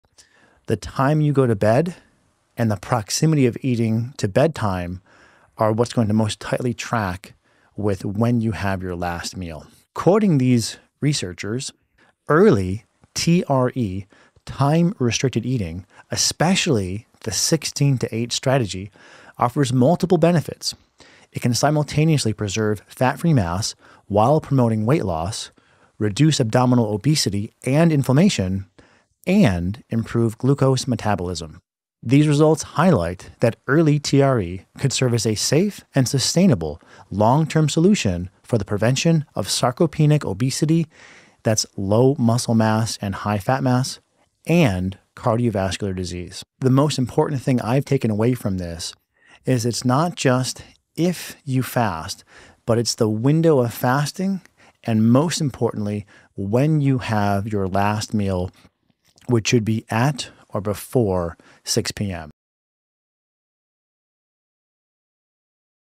The best fasting time (New study)
Meal timing may be one of the most overlooked strategies in metabolic health.

It’s not just about how long you fast — it’s about when you stop eating. Research on early time-restricted eating shows that aligning your meals with your circadian rhythm can support blood sugar regulation, reduce inflammation, and help preserve lean muscle.

➡️ For many people, simply shifting the last meal of the day earlier could be the missing piece to unlocking better energy, body composition, and long-term results.

I'm Dr. Ruscio, researcher and clinician at the Ruscio Institute.